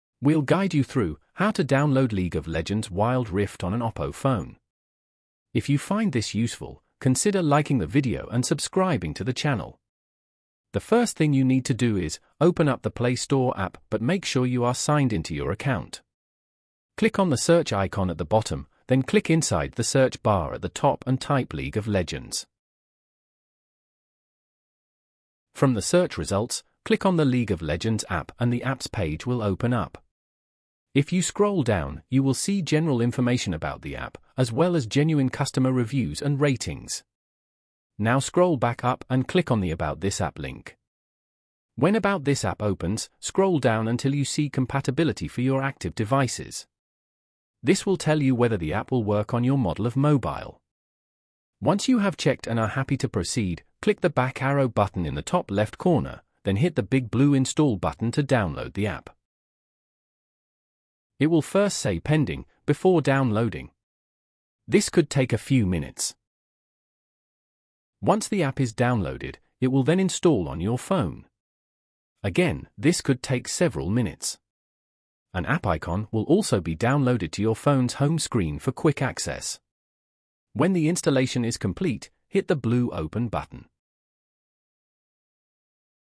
How To Download League Of Legends Wild Rift On Oppo Phone (Step By Step)
This video will show you how to download League of Legends Wild Rift on Oppo phone.